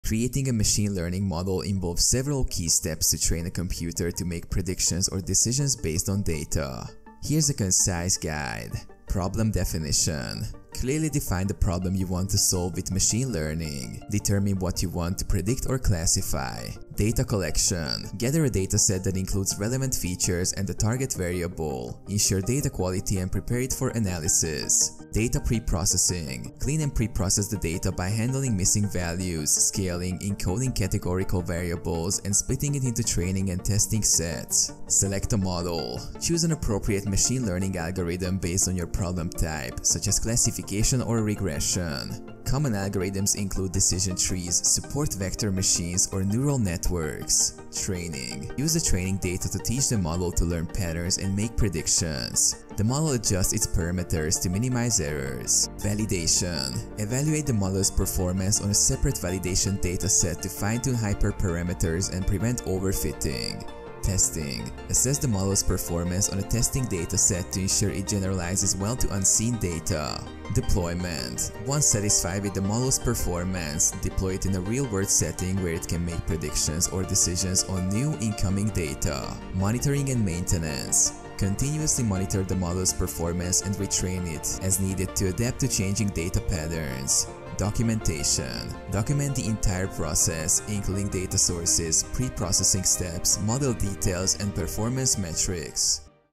How To Create A Machine Learning Model In 2024?
In this video I will explain How You Can Create A Machine Learning Model In 2024.